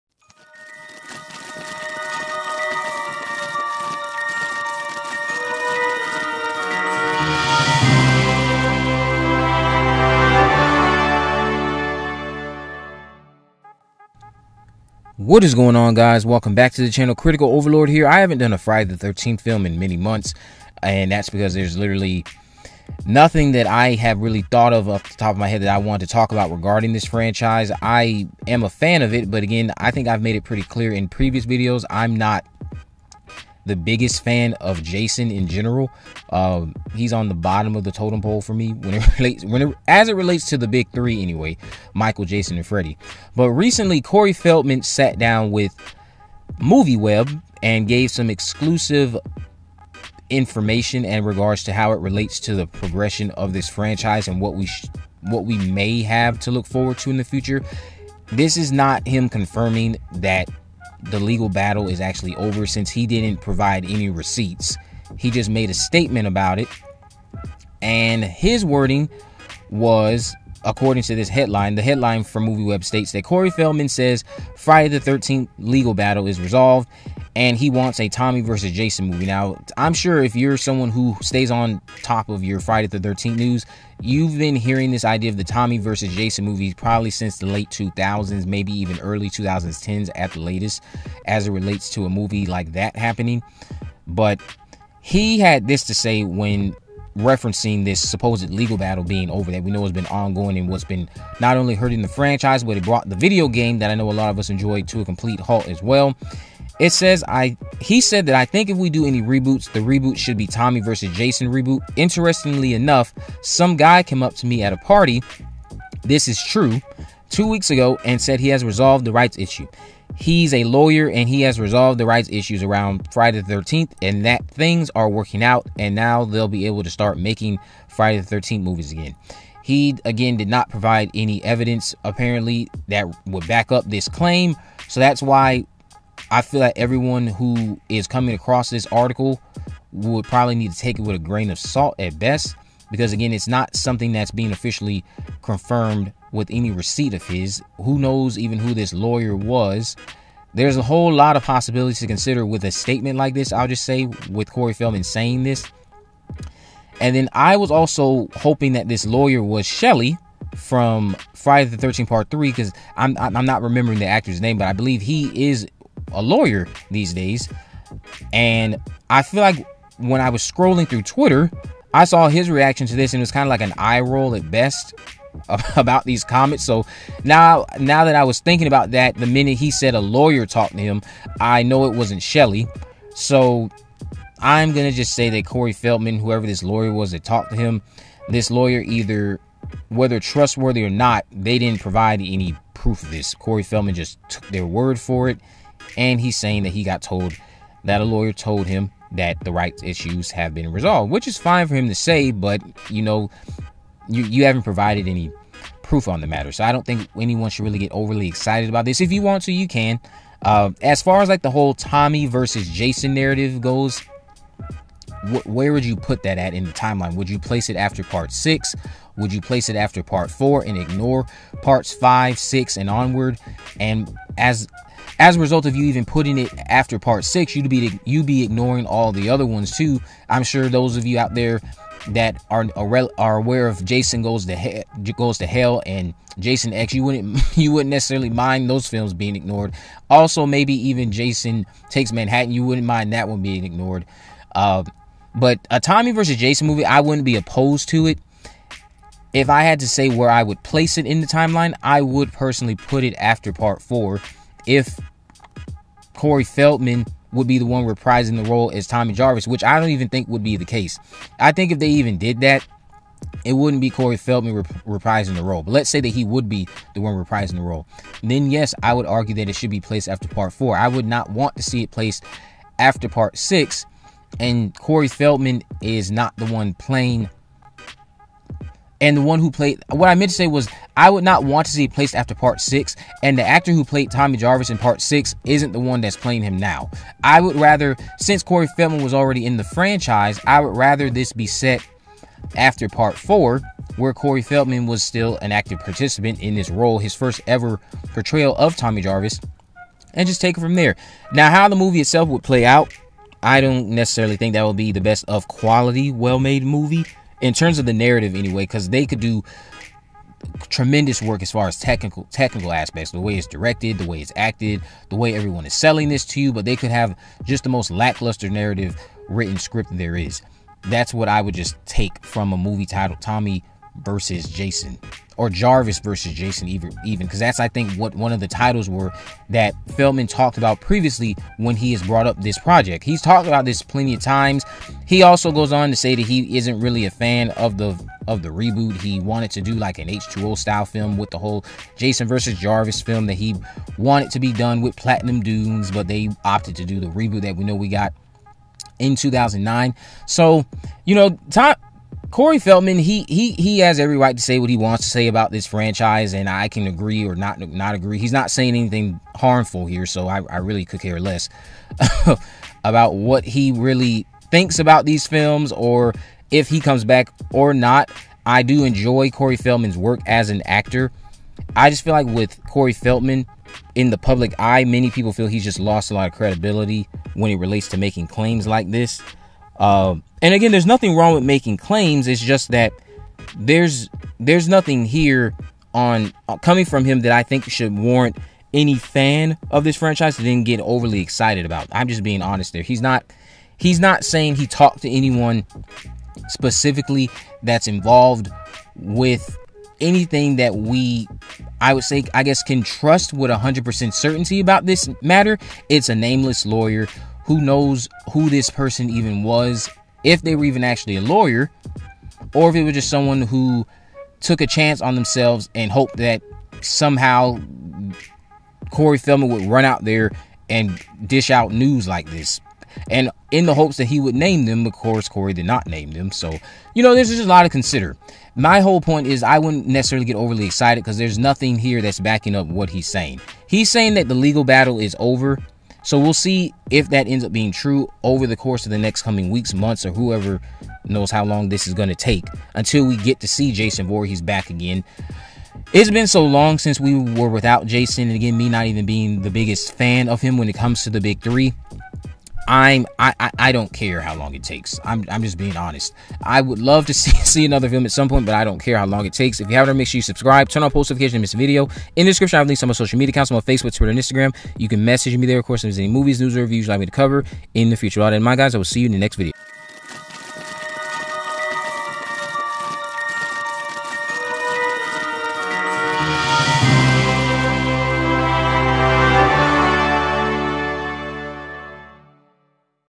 Friday the 13th | Corey Feldman Says Jason Voorhees Legal Trouble Over
Corey Feldman starred in Friday the 13th Part 4 as Tommy Jarvis and he claims the court struggles have been settled. An unnamed lawyer told him this, but that's all he said before going on about wanting to do a Jason vs Jarvis film. Will Jason Voorhees return very soon if these claims are true? What direction would the franchise even take at this point?